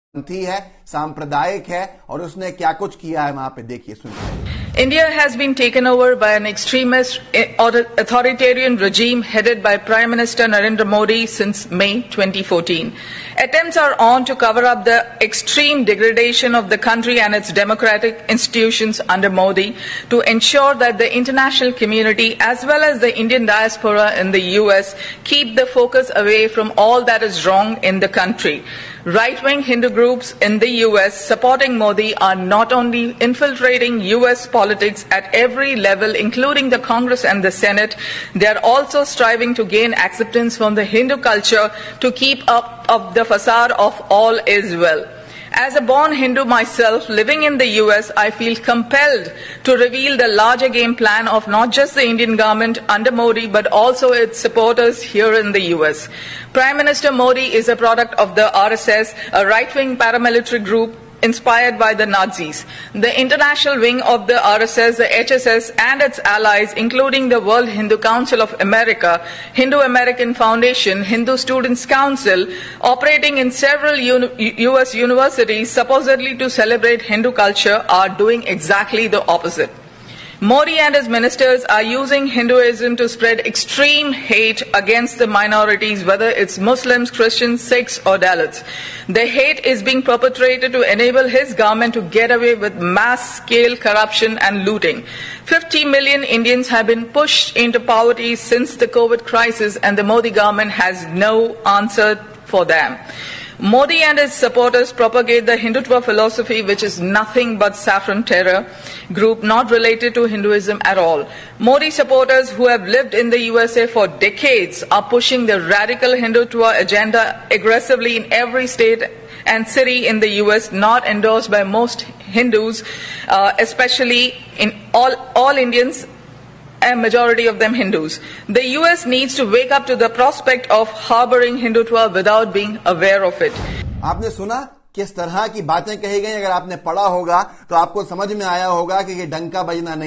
जो अंधभक्त कहते हैं मोदीजी का डंका बज रहा है विदेश में,वो ये एक बार सूनले की डंका हे या लंका
जो अंधभक्त कहते हैं मोदीजी का डंका बज रहा है विदेश में,
वो ये एक बार सूनले की डंका हे या लंका👇